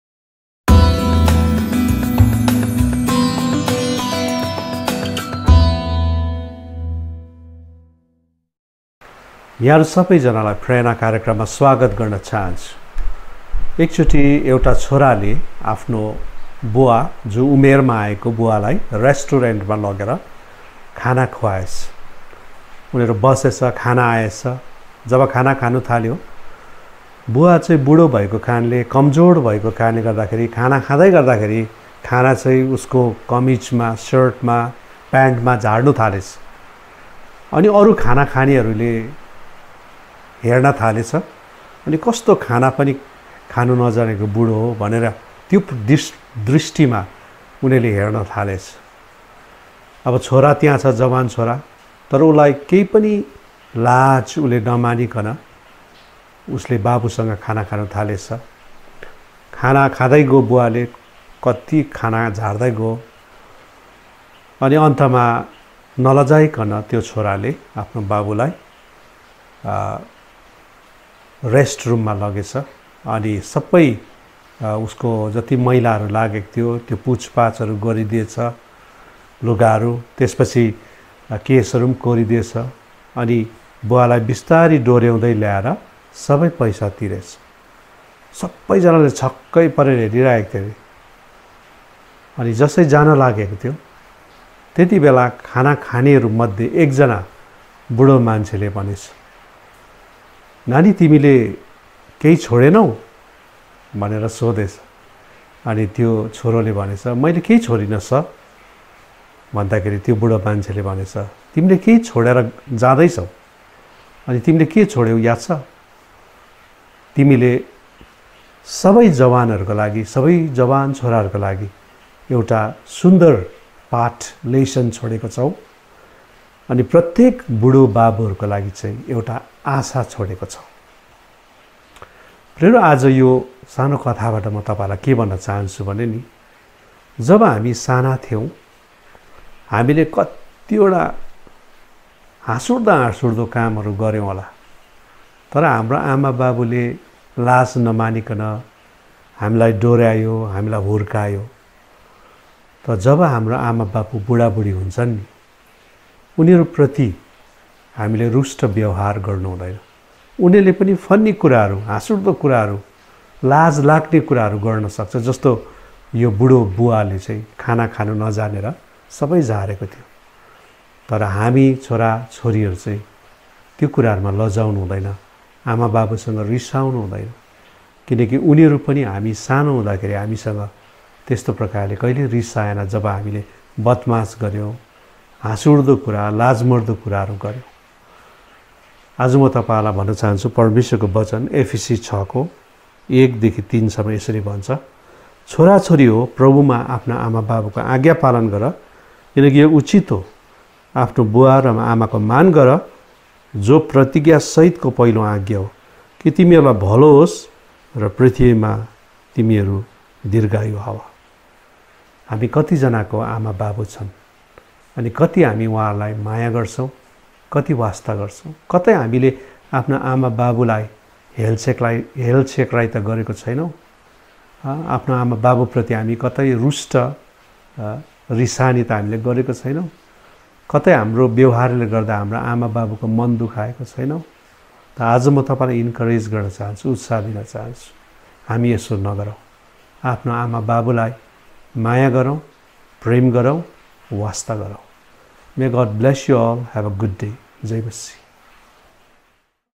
PRERNA : AN INSPIRATION (LOVE YOUR PARENTS आमा बुबालाई माया गर्नुहोस् I)
LOVE YOUR PARENTS:आमा बुबा लाई माया गर्नुहोस् I
PRERNA - is to inspire you all every morning to live your life in fullest, with positive attitude and intimacy with God to understand that your life is a precious gift of God.